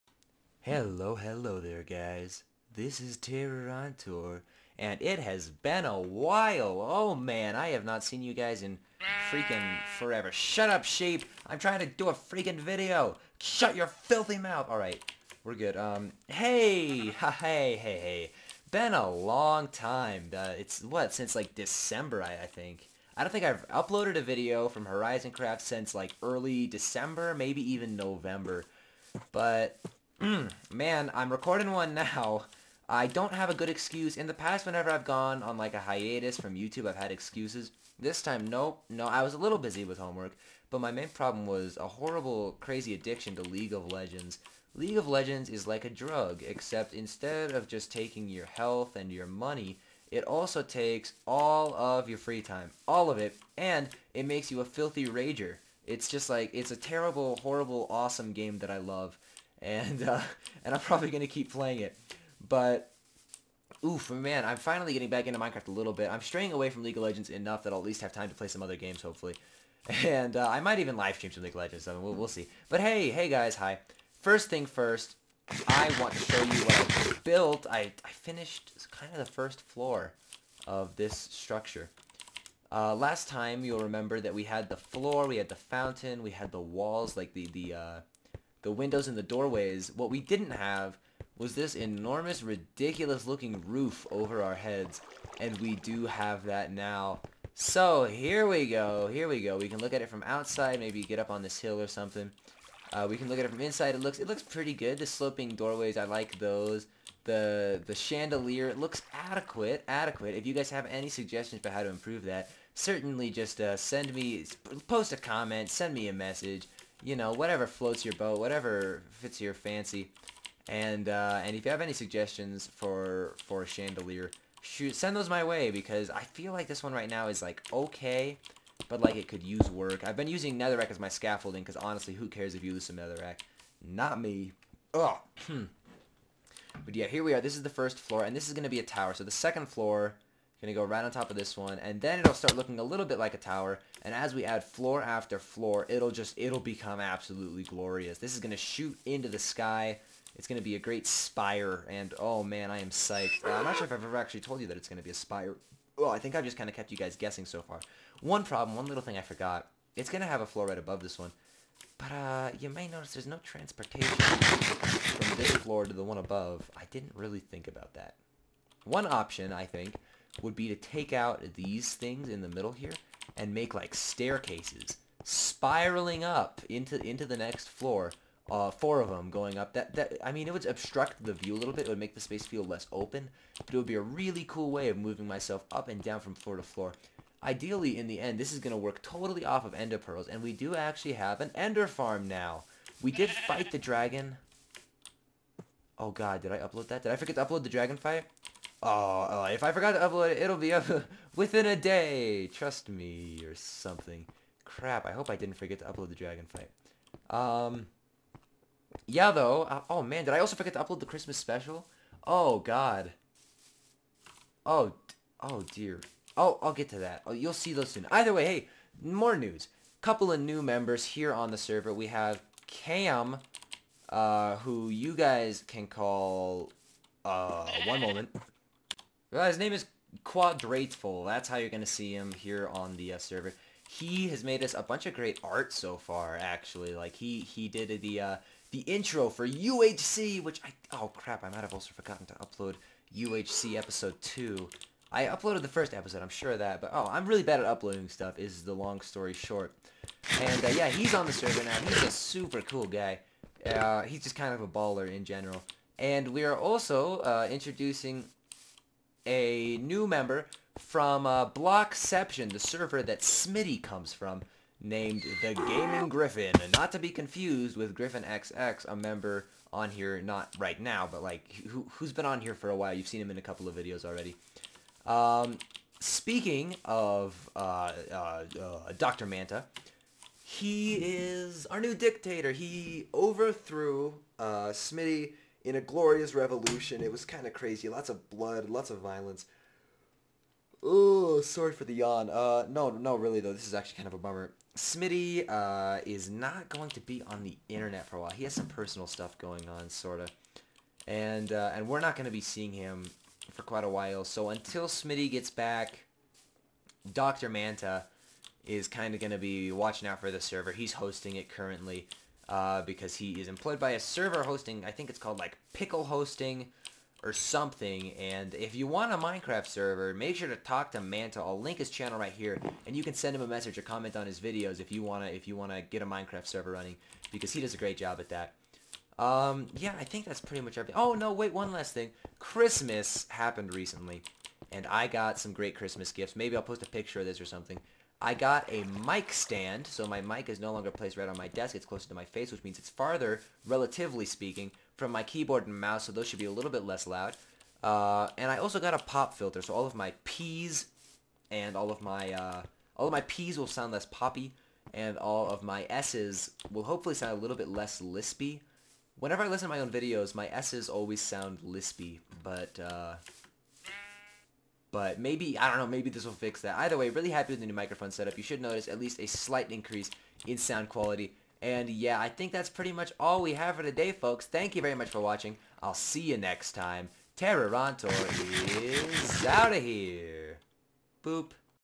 HorizonCraft Episode 6
Hey guys. It's been a while, and as you can see, I've made considerable progress! I actually recorded some episodes between Episode 5 and this one, but I never got around to uploading them and now they're lost forever. Thanks for sticking around!